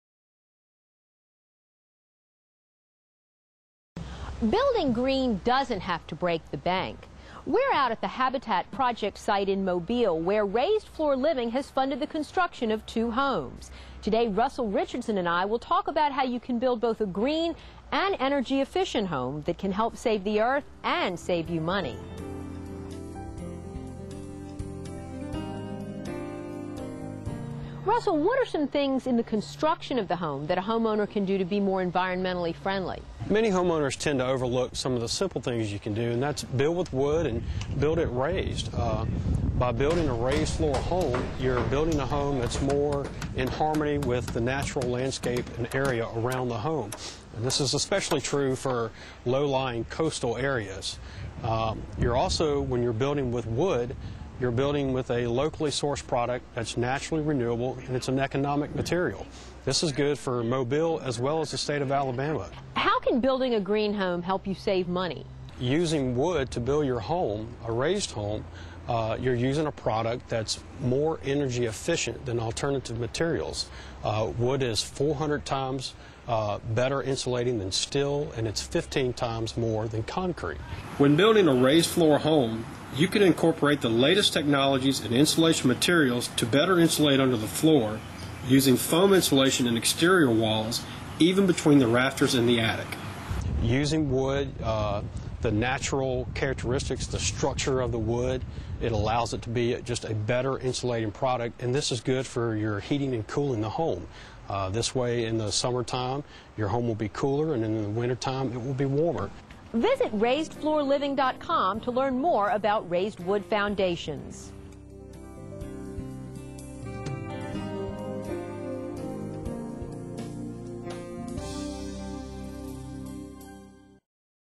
Raised Floor Living in Mobile -- Green Building & Energy Efficiency
Raised Floor Living and Habitat for Humanity in Mobile County team up to compare the costs and advantages of building homes with raised wood floor foundations rather than concrete slab foundations.